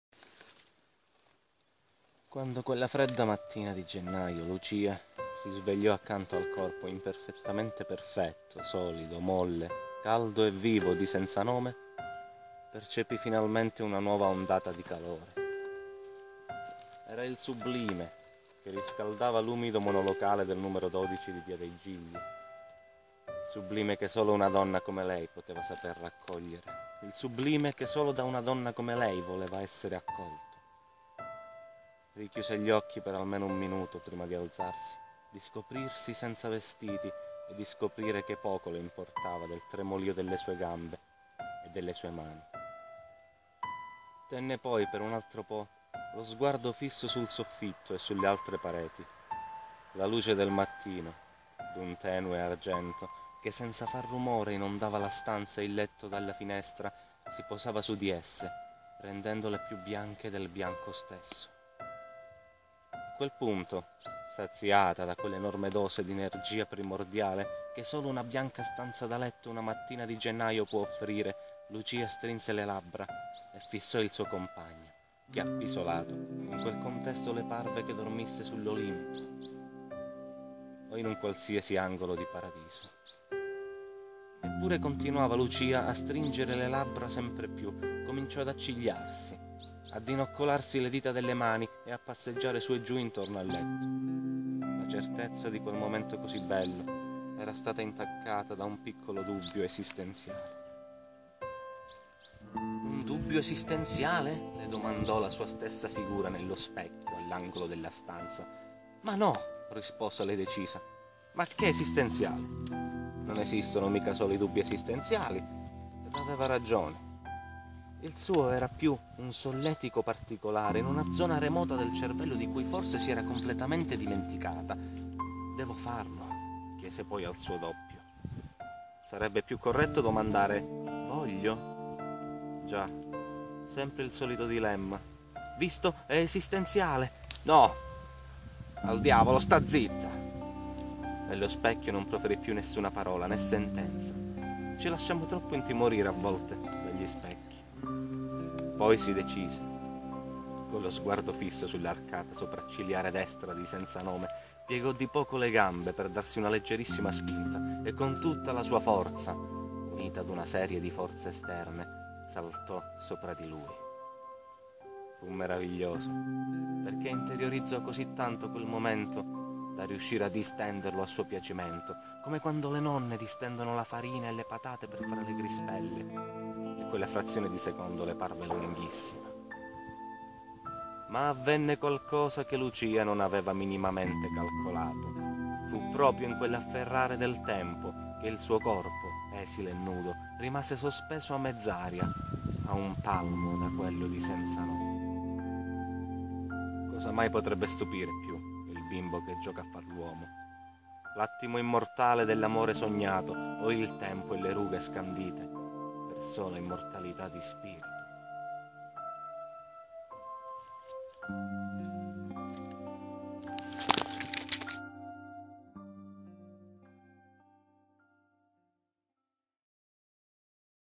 Il salto di Lucia - 3. Sospensione
[Vi racconto di quando Lucia decise di saltare sul corpo di Senzanome, una fresca mattina di gennaio in un umido monolocale al numero dodici di Via dei Gigli, rimanendo sospesa a mezz'aria e raggiungendo l'eternità]